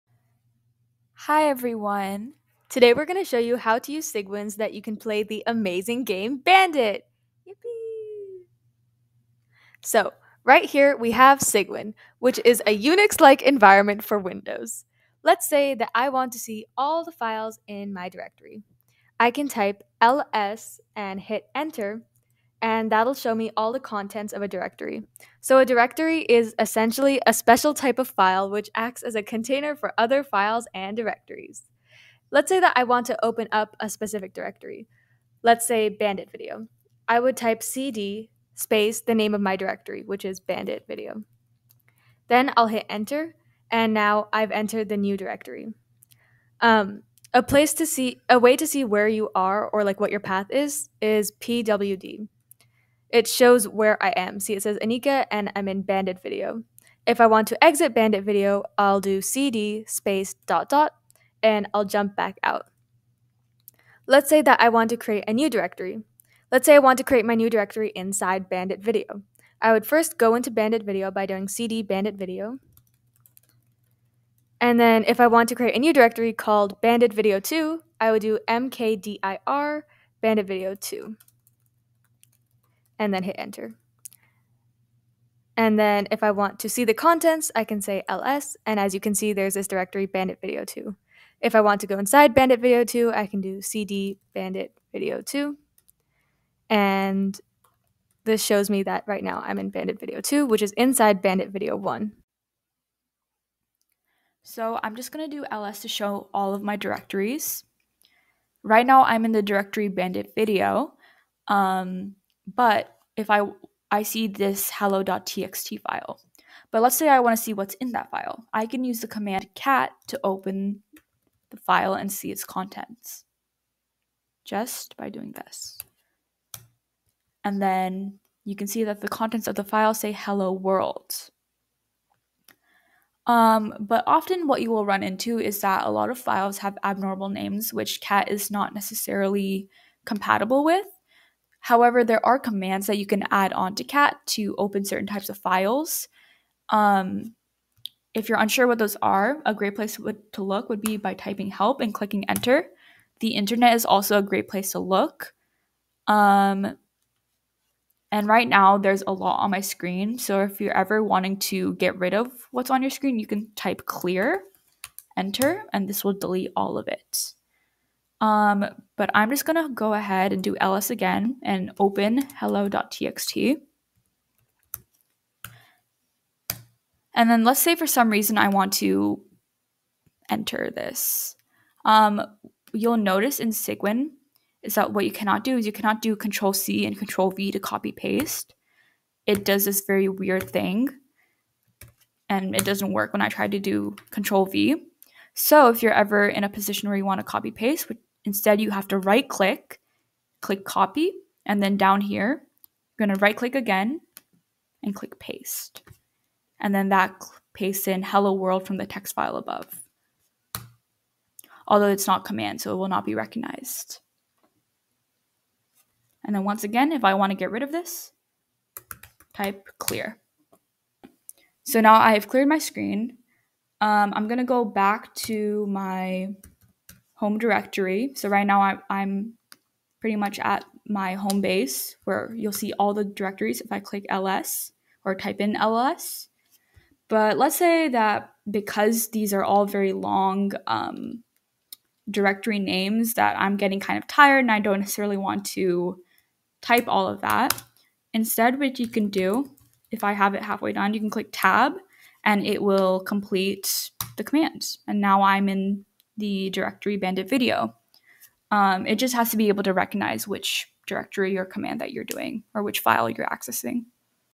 Basic Unix commands using Cygwin (for Bandit)
Mahi and Anika walk through important commands for moving around in a Unix console. We are using Cygwin, which is a Unix emulator for Windows systems.